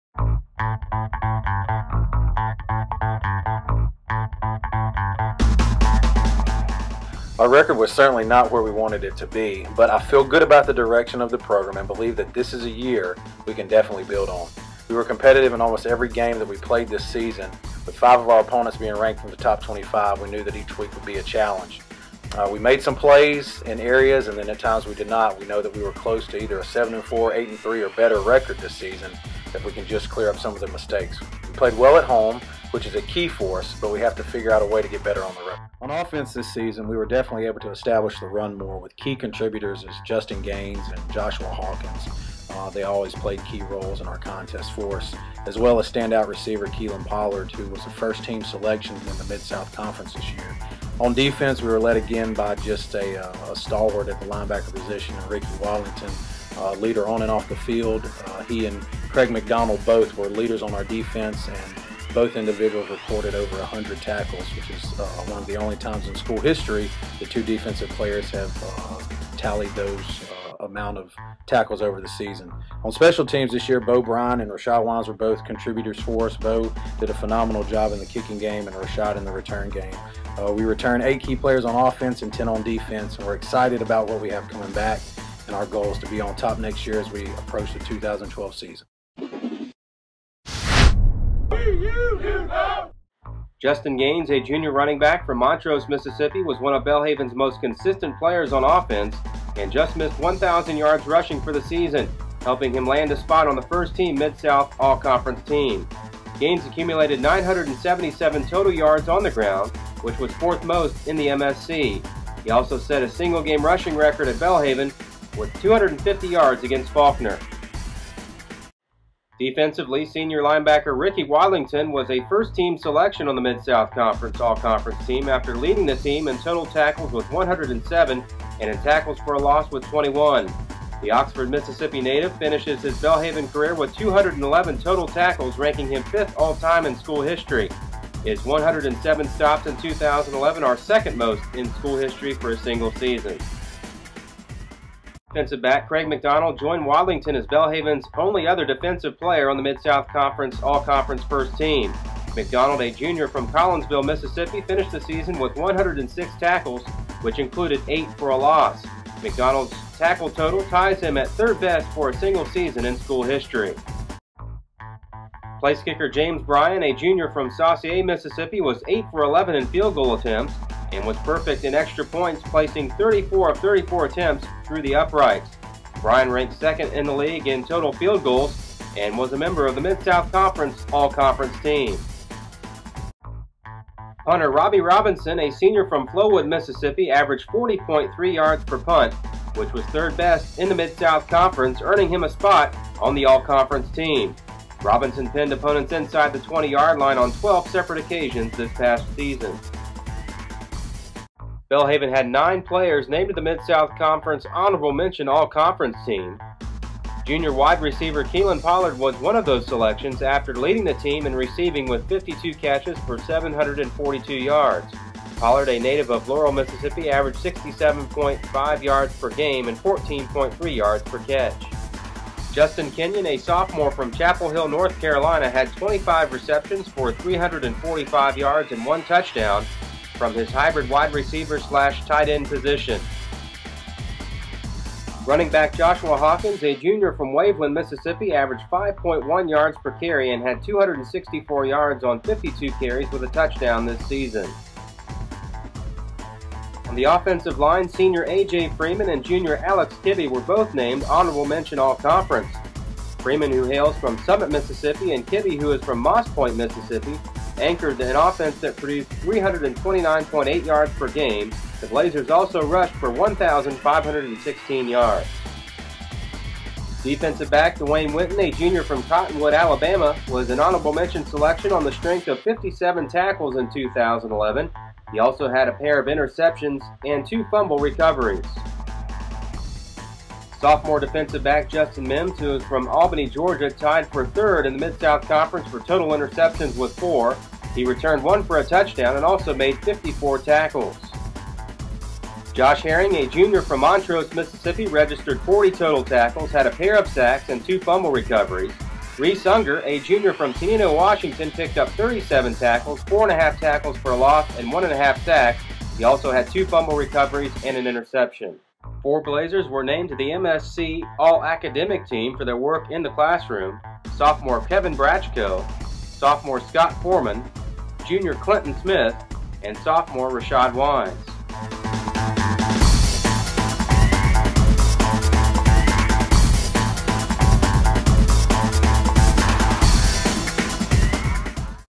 2011 Belhaven Football Highlights
The Belhaven football team experienced both highs and lows during the 2011 campaign that concluded with a 5-6 overall record and 3-3 mark in the Mid-South Conference West Division. The Blazers improved on their 4-7 record from a season ago and just missed on some opportunities to pick up a couple of more wins during the season. Belhaven played extremely well at home going 5-1 at H.T. Newell Field, with the programs first ever victory against Mississippi College. There are certainly some big positives to build on as the Blazers look ahead to the 2012 season.